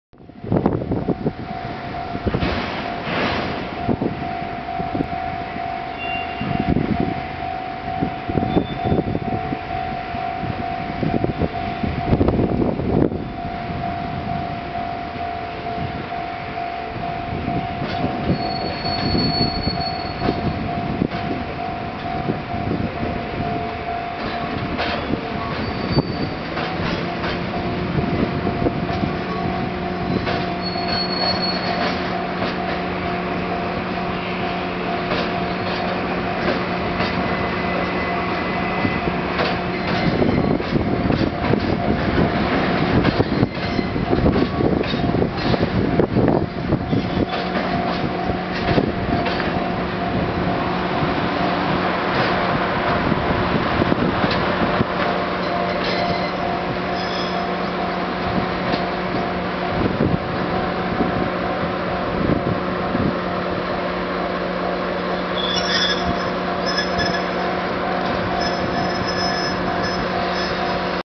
ＪＲ関西本線 四日市駅 ＤＤＤＥタキ入換中
ＤＤ５１８５２とＤＥ１０１７２５　重連非総括運転中